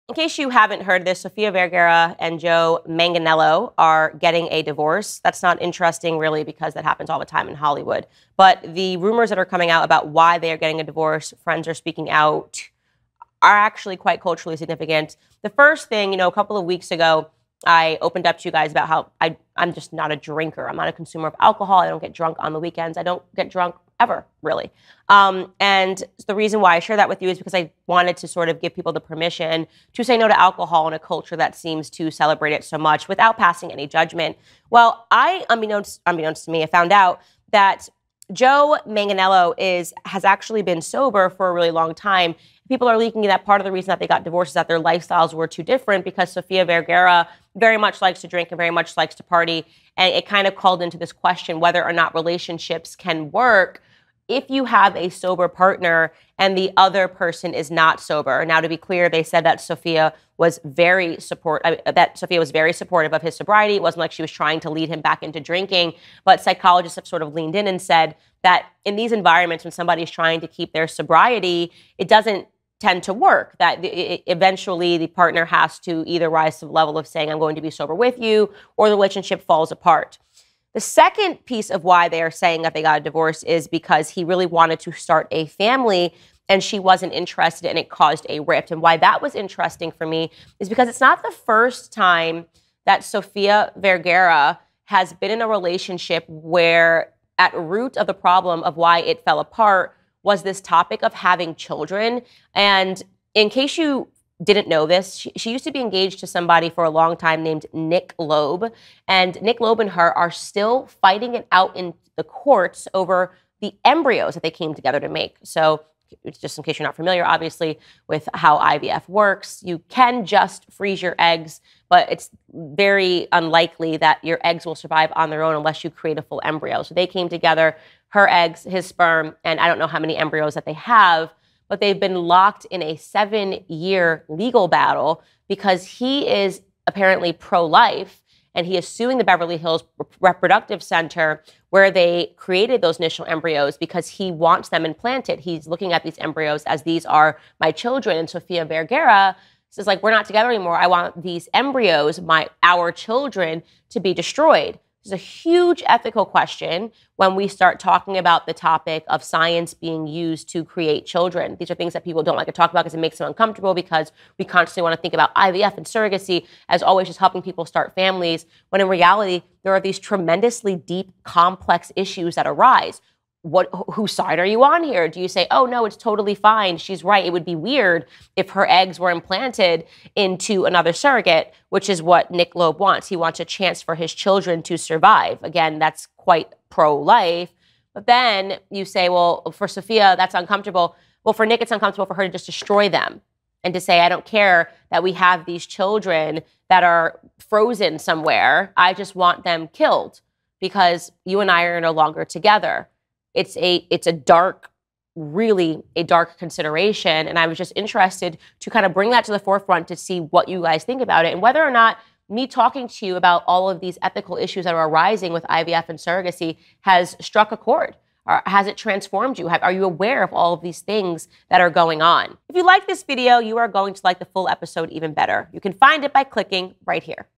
Is THIS The Reason for Sofia Vergara and Joe Manganiello’s Divorce?!
Sofia Vergara and Joe Manganiello have announced that they are getting a divorce. This is not new in Hollywood, but the rumors surrounding the divorce are very culturally relevant. Let's talk about them.

Watch “The Greatest Lie Ever Sold: George Floyd and the Rise of BLM” exclusively on DailyWire+